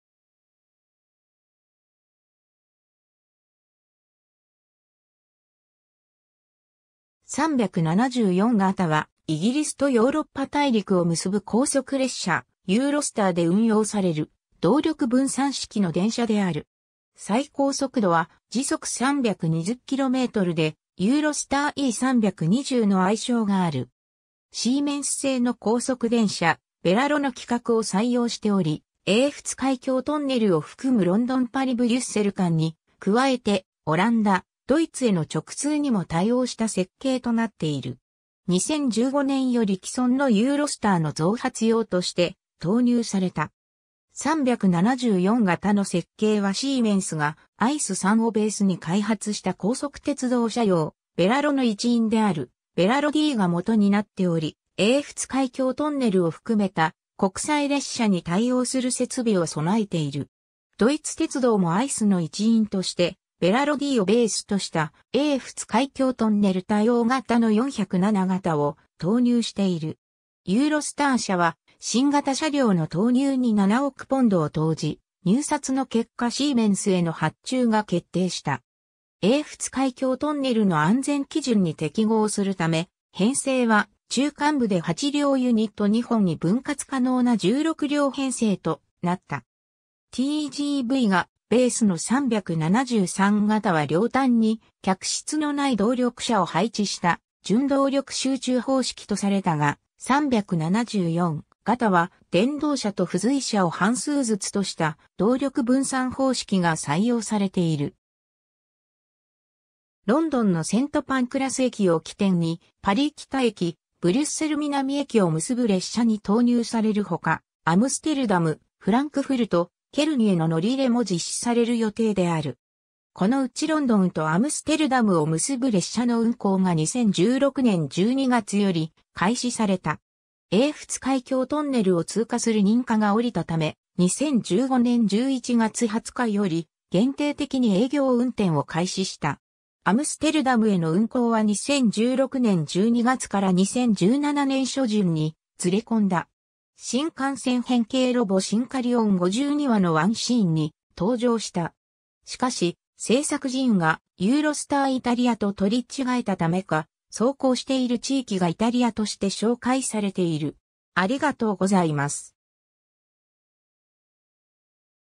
イギリス鉄道374形電車
374形（Class 374）は、イギリスとヨーロッパ大陸を結ぶ高速列車ユーロスターで運用される動力分散式の電車である。
最高速度は320km/hで、「ユーロスターe320」の愛称がある。
シーメンス製の高速電車「ヴェラロ」の規格を採用しており、英仏海峡トンネルを含むロンドン・パリ・ブリュッセル間に加えてオランダ、ドイツへの直通にも対応した設計となっている。
2015年より既存のユーロスターの増発用として投入された。
 374形の設計はシーメンスがICE 3をベースに開発した高速鉄道車両「ヴェラロ」の一員であるヴェラロDが基になっており、英仏海峡トンネルを含めた国際列車に対応する設備を備えている。
ドイツ鉄道もICEの一員としてヴェラロDをベースとした英仏海峡トンネル対応型の407形を投入している。
 ユーロスター社は新型車両の投入に7億ポンドを投じ、入札の結果シーメンスへの発注が決定した。
英仏海峡トンネルの安全基準に適合するため、編成は中間部で8両ユニット2本に分割可能な16両編成となった。
TGVがベースの373形は両端に客室のない動力車を配置した準動力集中方式とされたが、374形は電動車と付随車を半数ずつとした動力分散方式が採用されている。
 ロンドンのセント・パンクラス駅を起点にパリ北駅、ブリュッセル南駅を結ぶ列車に投入されるほか、アムステルダム、フランクフルト、ケルンへの乗り入れも実施される予定である。
このうちロンドンとアムステルダムを結ぶ列車の運行が2016年12月より開始された。
 英仏海峡トンネルを通過する認可が降りたため、2015年11月20日より限定的に営業運転を開始した。
アムステルダムへの運行は2016年12月から2017年初旬にずれ込んだ。
 「新幹線変形ロボ シンカリオン」 52話のワンシーンに登場した。
しかし、製作陣がユーロスターイタリアと取り違えたためか、走行している地域がイタリアとして紹介されている。